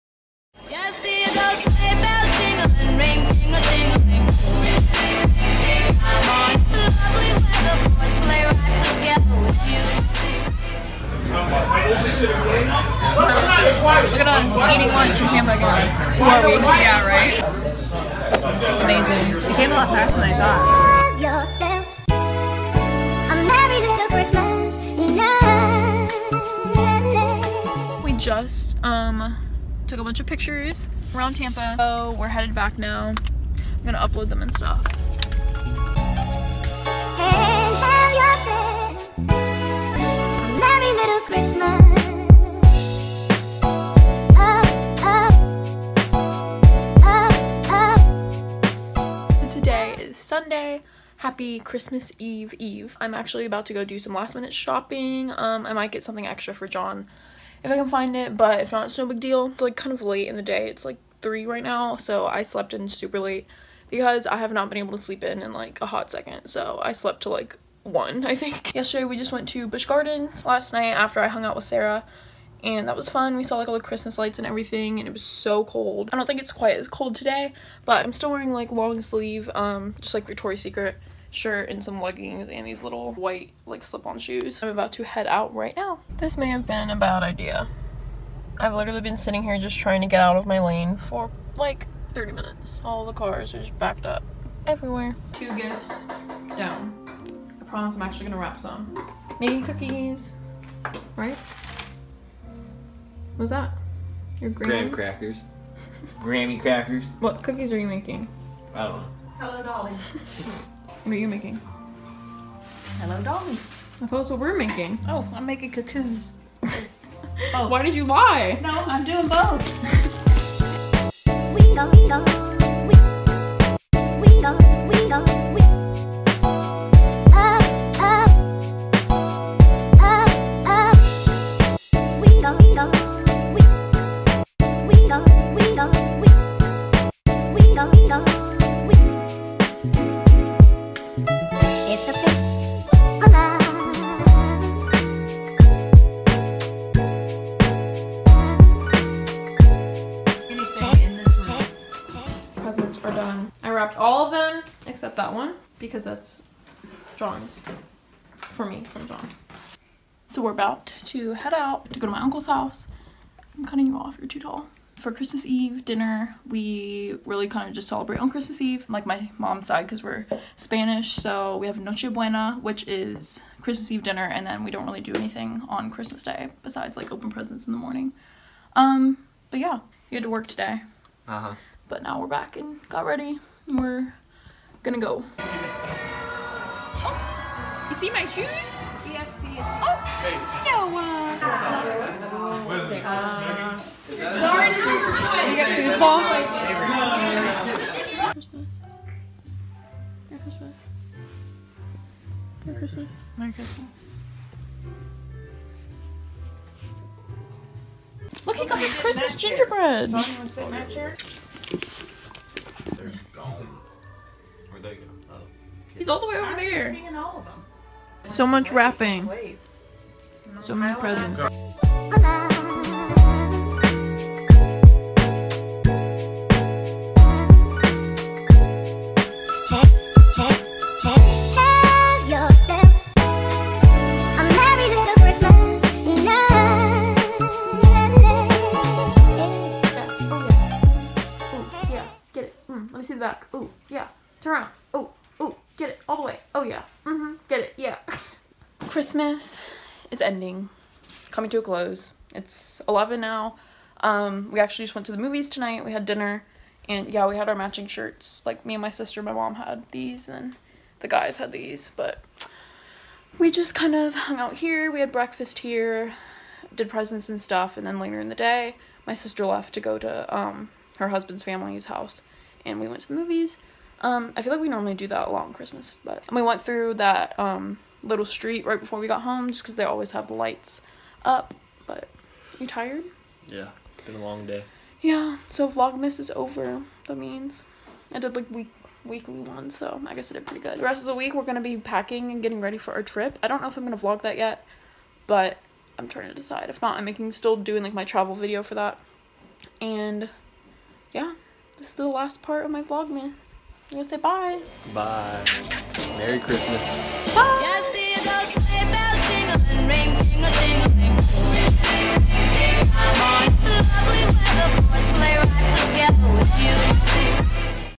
VLOGMAS WEEK 4 ✩ merry christmas!!
thank you for keeping up with me this winter break during vlogmas!! i hope you all enjoy this last short little vlog of christmas eve and christmas day with my family :)